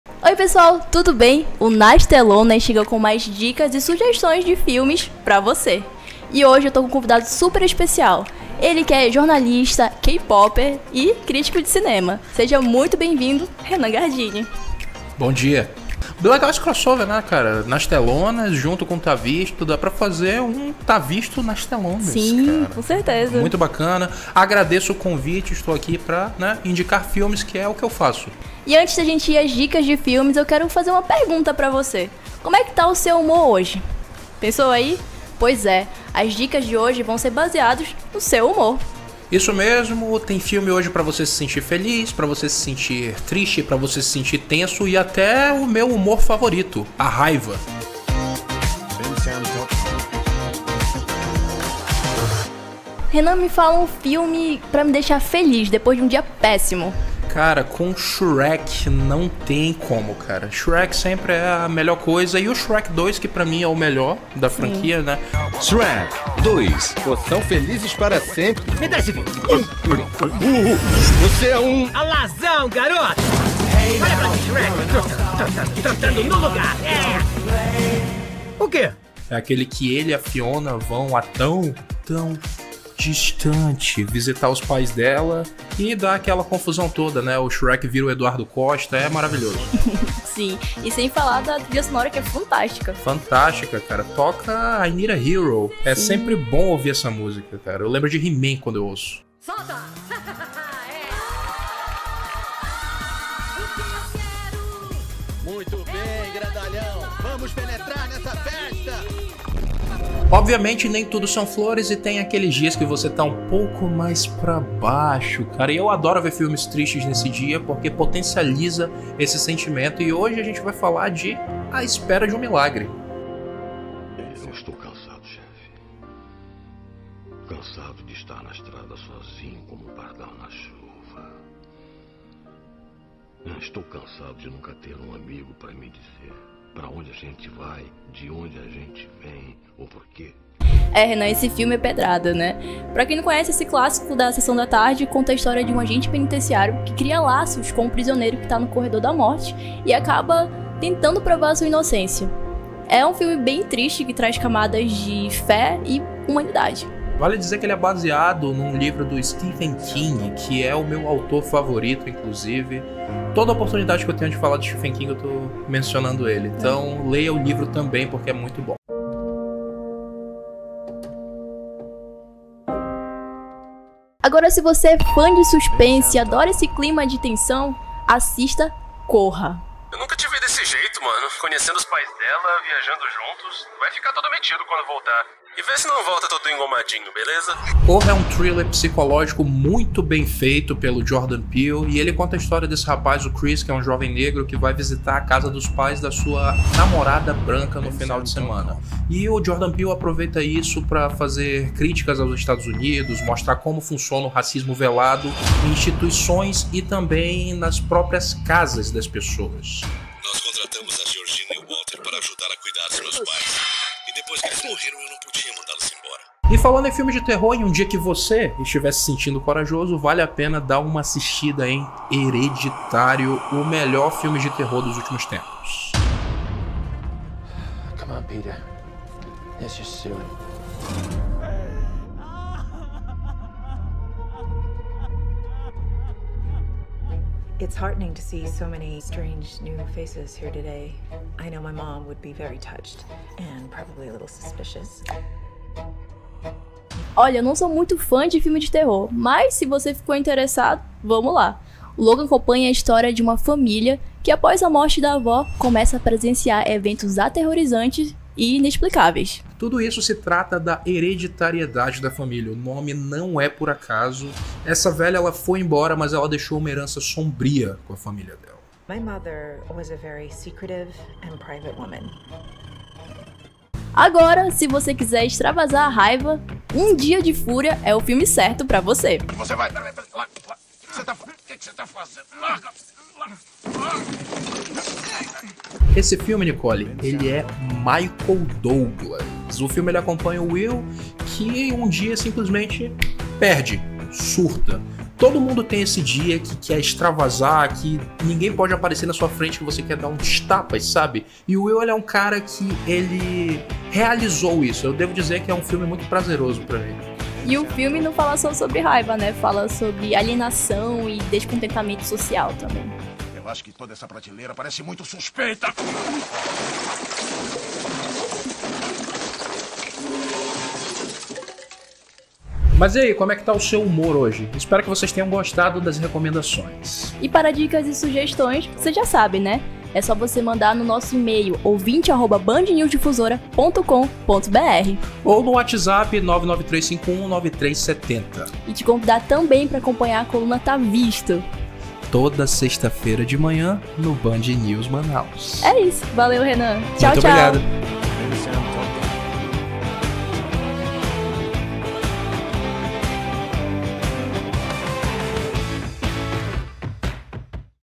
Filmes que mexem com o seu humor
Os filmes têm o poder de nos transportar para mundos diferentes e evocar uma ampla gama de emoções.

Nesse crossover de "Nas Telonas" e "Ta Visto!", Nicoly Nogueira e Rennan Gardini, prometem uma lista de filmes que vão mexer com o humor com os espectadores na salinha de cinema.

Confira!